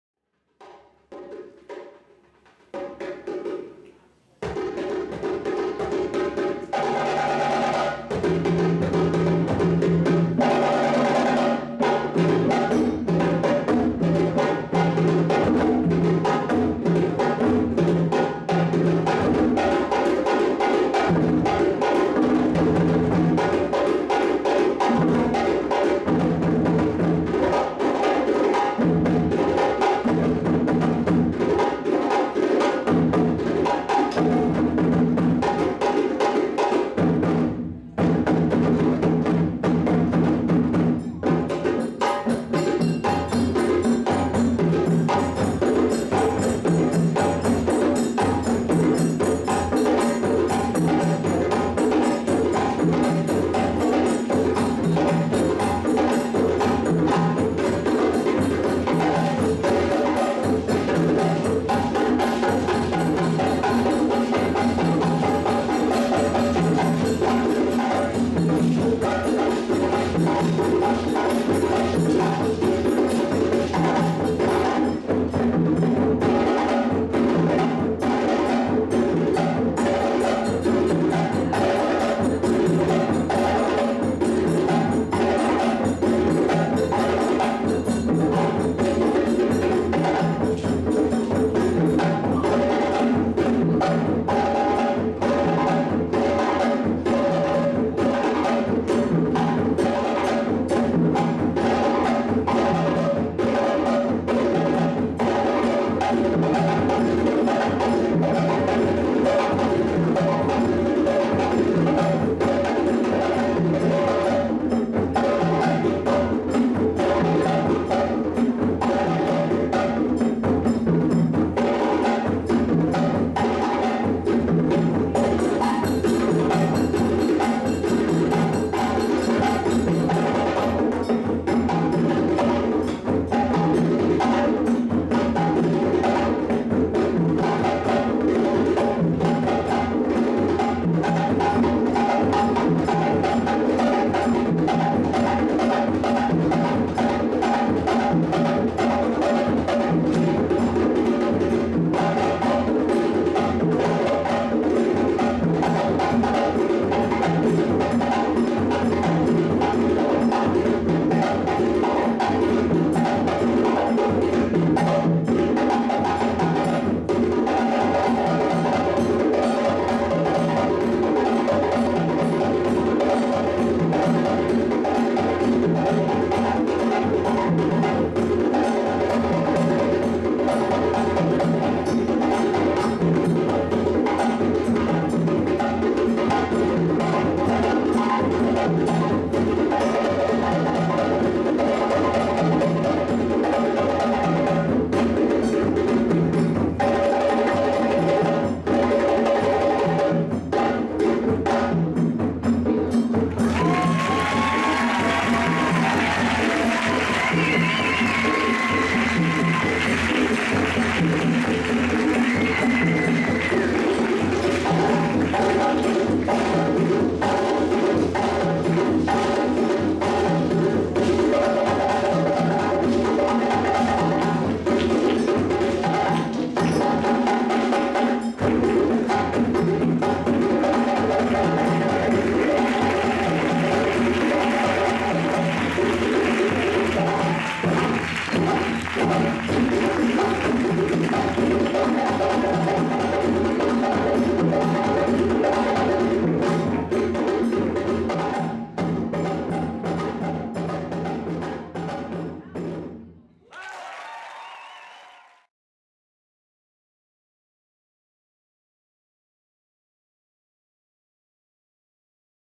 Les Guinées - Dansem per Filipines
Gala benéfica "Dansem per Filipines"  organizada por Noemí Martínez.
Todos los beneficios han sido para ayudar a la reconstrucción de un orfanato en la zona devastada por el tifón Yolanda, en Leyte, Filipinas, en noviembre del 2013.

Les Guinées grupo de danza malinké.